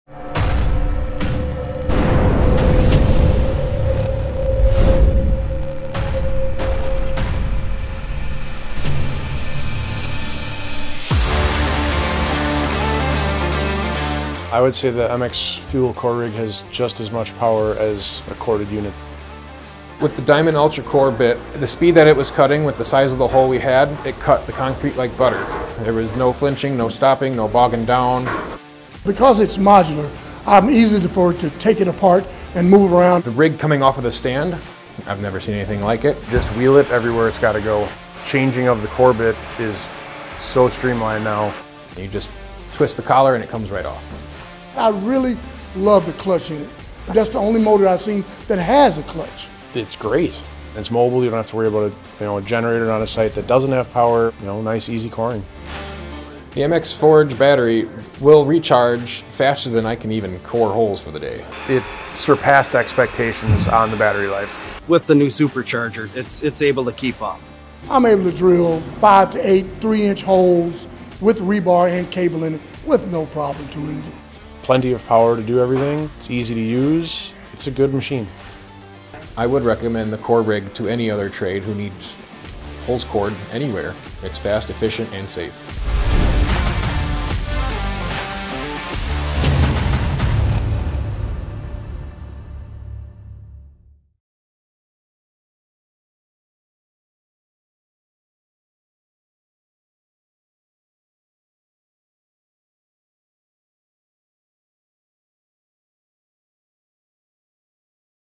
MX FUEL™ Core Rig w/ Stand
Our MX FUEL™ Core Rig w/ Stand delivers unmatched productivity with the power to core up to 14” holes in reinforced concrete, the easiest coring anywhere, and four 10” holes through reinforced concrete per charge. The POWERSTATE™ Brushless Motor delivers the power to complete heavy-duty applications, coring up to 14” holes in reinforced concrete. REDLINK PLUS™ Intelligence ensures maximum performance and protection from overload, overheating, and over-discharge. The intuitive bit removal device, 3-piece modular design, and elimination of a cord maximize productivity, providing you with the easiest coring experience onsite. The MX FUEL™ Core Rig w/ Stand is kitted with two (2) MX FUEL™ REDLITHIUM™ FORGE™ HD12.0 batteries & a Super Charger. In just 65 minutes, you can charge an MX FUEL™ REDLITHIUM™ FORGE™ HD12.0 to 100%. Both the MX FUEL™ REDLITHIUM™ FORGE™ HD12.0 Battery & MX FUEL™ Super Charger are COOL-CYCLE™ Capable. COOL-CYCLE™ Active Cooling System provides high-speed cooling for less downtime. The active cooling feature works with MX™ batteries that have COOL-CYCLE™ capability. The core rig is compatible with all MX FUEL™ REDLITHIUM™ batteries, giving you the hardest working and most reliable power for equipment. The MILWAUKEE® MX FUEL™ Core Rig w/ Stand is ONE-KEY™ compatible so you can track, manage, and secure your equipment.